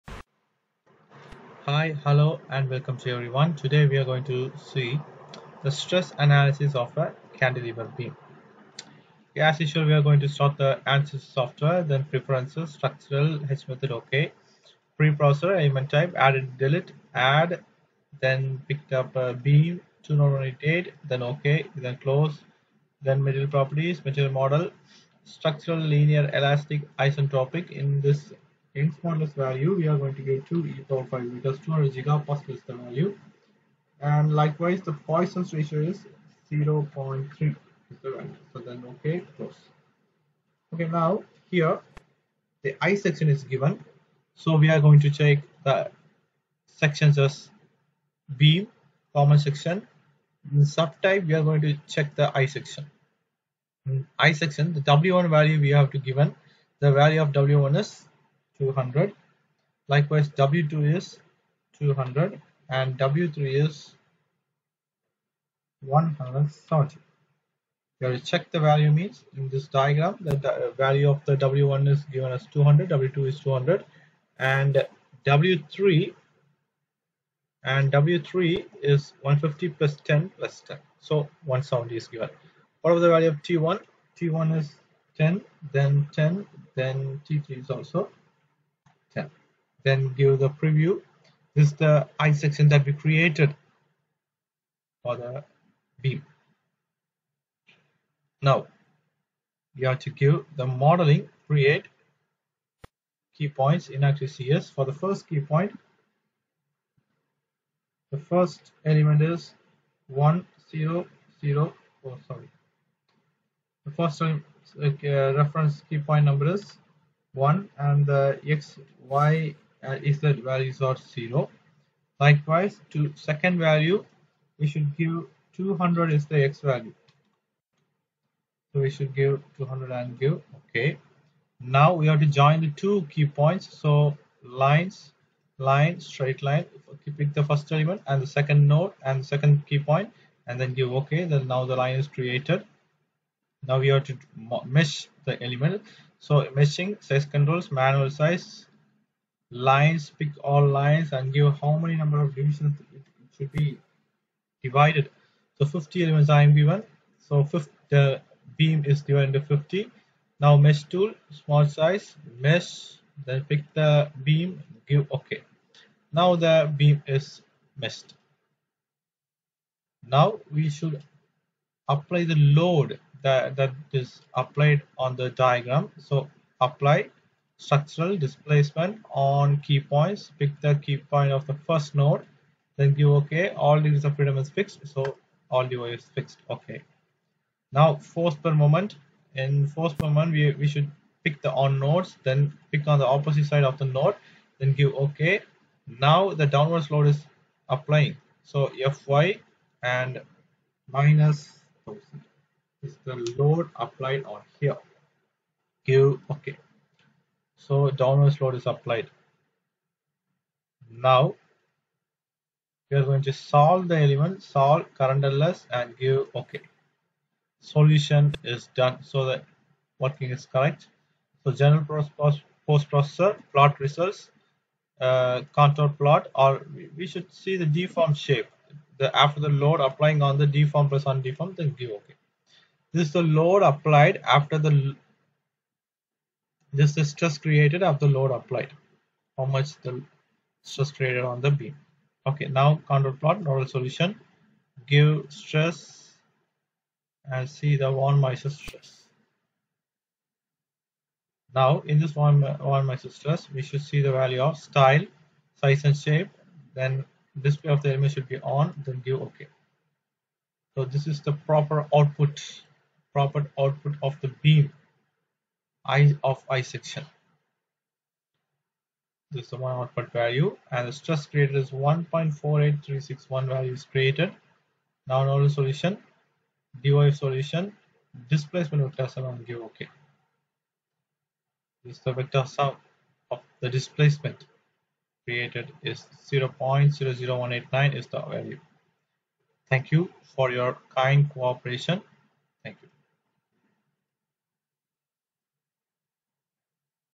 ANSYS : Stress analysis of a cantilever beam
Outcome:
Able to analyse a cantilever beam using the ANSYS software.

Application:
1. Structural  Application
2. Construction Application